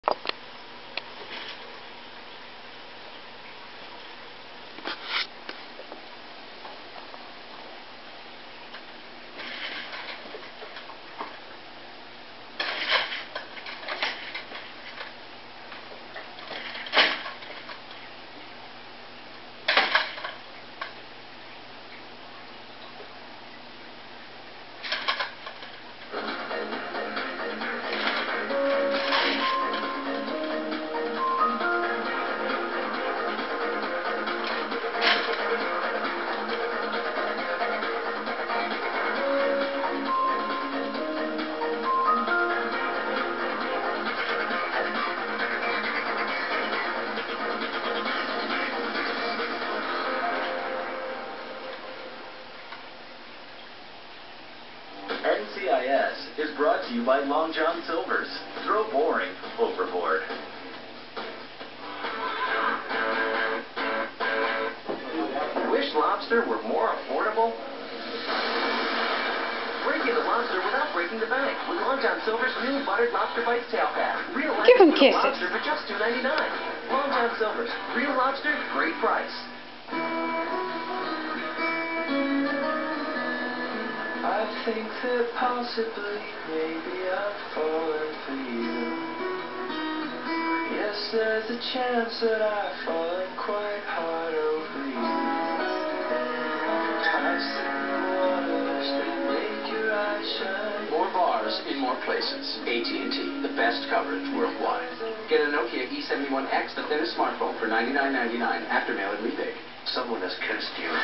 Wyatt and rattie playtime
Wyatt has started interacting with the ratties. He is still afraid of them when they run towards him. Nothing is funnier than watching a 74lb dog jump in the air afraid of a tiny rat